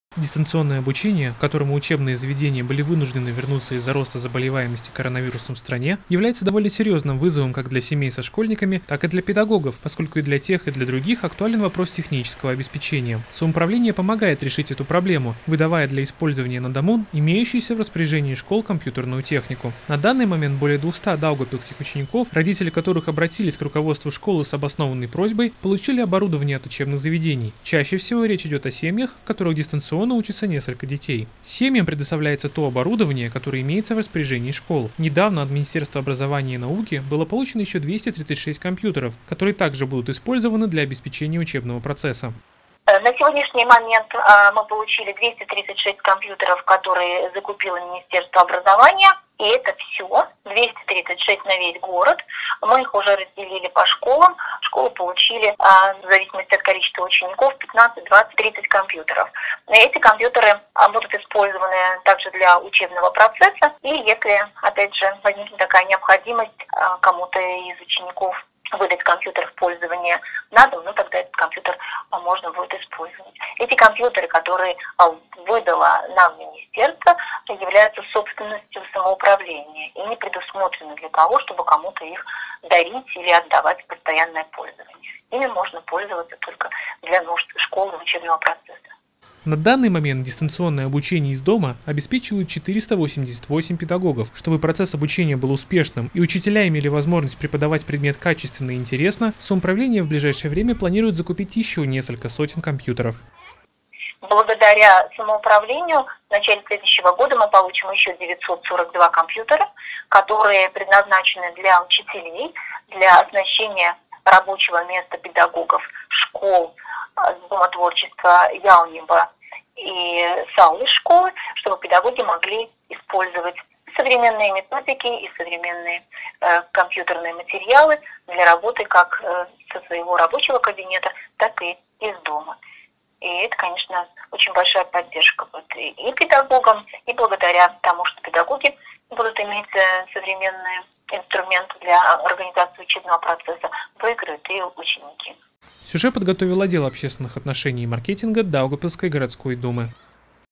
Самоуправление помогает школьникам и учителям решить вопрос технического обеспечения
Дистанционное обучение, к которому учебные заведения были вынуждены вернуться из-за роста заболеваемости в стране, является довольно серьезным вызовом как для семей, в которых есть школьники, так и для педагогов, поскольку и для тех, и для других актуален вопрос технического обеспечения. Самоуправление помогает решить эту проблему, выдавая для использования на дому имеющуюся в распоряжении школ компьютерную технику.
На данный момент более 200 даугавпилсских учеников, родители которых обратились к руководству школы с обоснованной просьбой, получили оборудование от учебных заведений. Чаще всего речь идет о семьях, в которых  несколько детей, учащихся дистанционно.